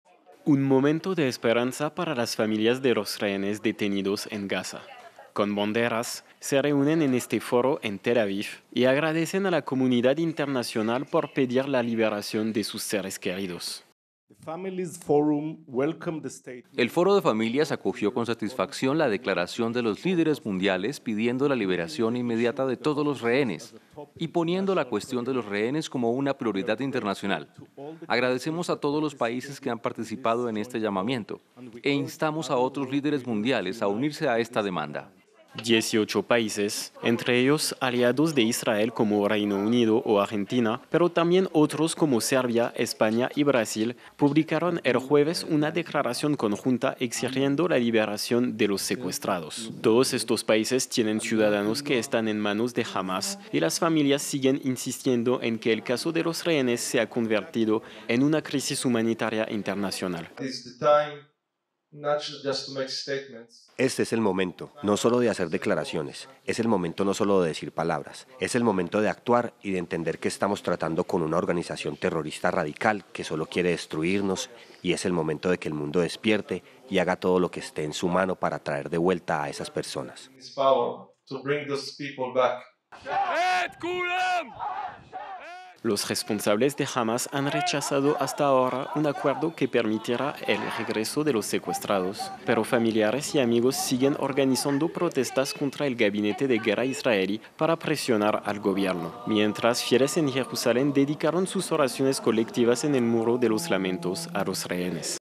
18 países piden la liberación de los rehenes que siguen en poder de Hamás
En un foro en Tel Aviv, familiares de los rehenes agradecieron a la comunidad intermacional que ha pedido la liberación de sus seres queridos, que siguen en poder de Hamás más de 200 días después de haber sido secuestrados. A pesar de las negativas del grupo islamista palestino, los israelíes hacen presión al gabinete de guerra para que siga negociando la liberación de los rehenes.